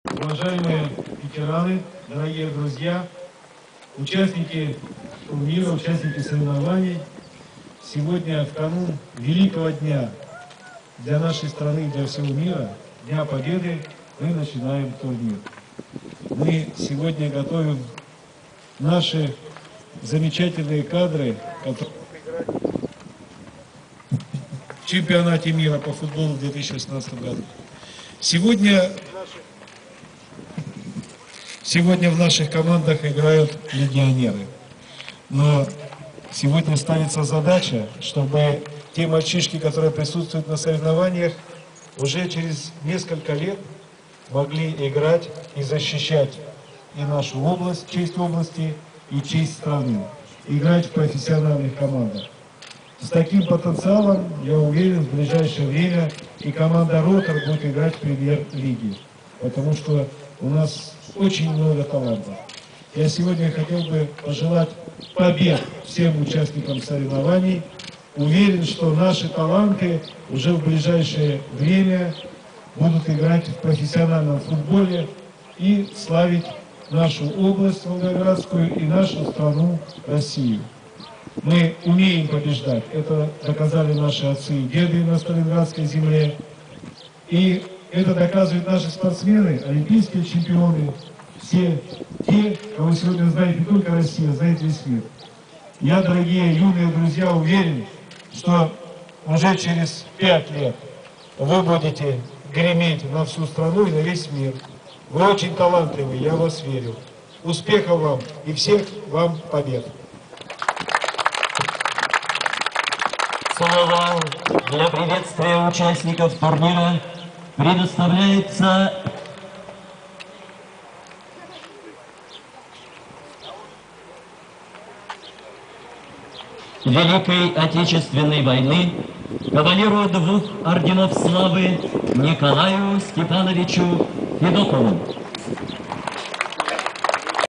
Сергей Боженов на открытии Кубка губернатора по футболу
На тренировочной базе «Ротор» 8 мая стартовал первый из трех турниров по футболу среди детских команд на Кубок губернатора Волгоградской области, посвященных 70-летию Победы в Сталинградской битве. Торжественная церемония открытия, в которой принял участие губернатор Сергей Боженов, состоялась у мемориального комплекса «Остров Людникова».